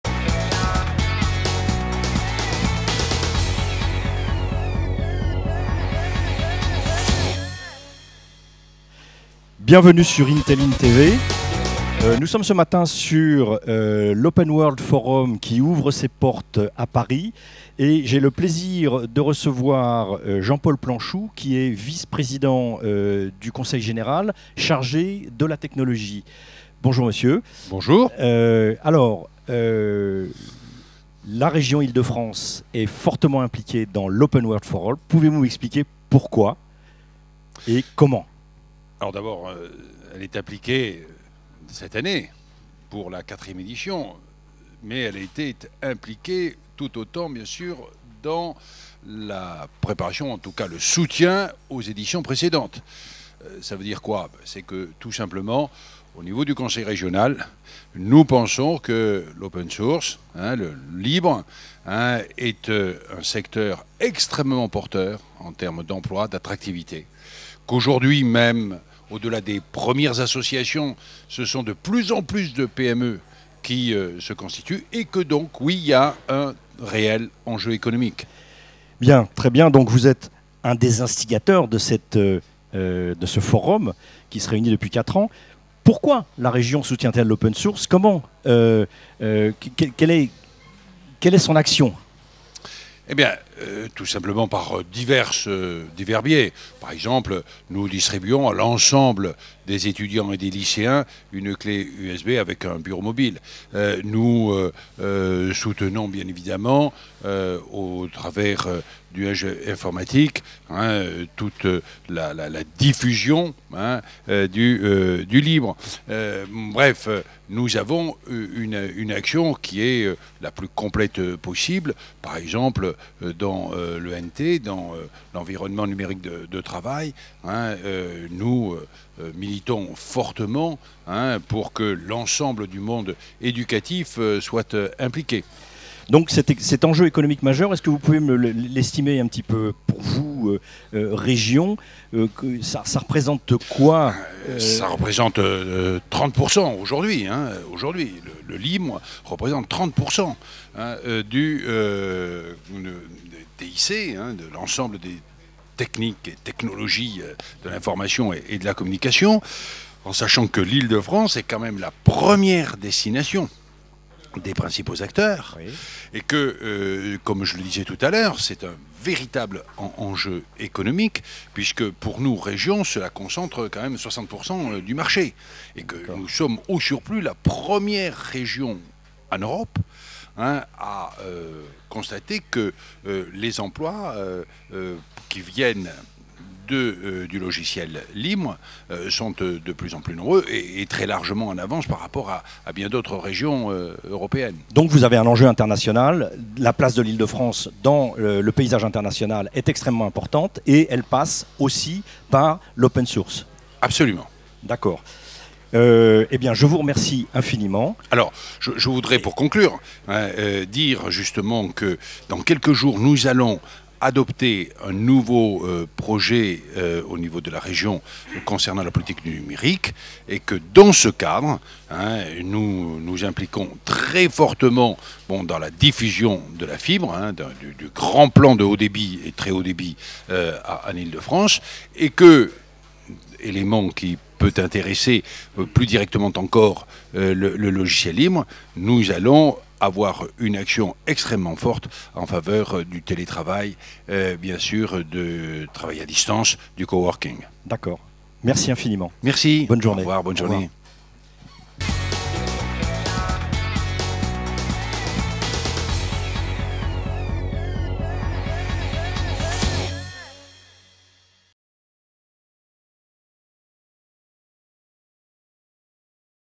La région Ile de France et l'open source par Jean-Paul Planchou
La région Ile de France et l'open source par Jean-Paul Planchou vice-président du Conseil régional d'Île-de-France, chargé du développement économique, de l'emploi, des Nouvelles technologies de l'information et de la communication, du tourisme, de l'innovation et de l'Économie sociale et solidaire. Open World Forum 2011, il est interviewé par Alain Baritault , Intelli'N TV.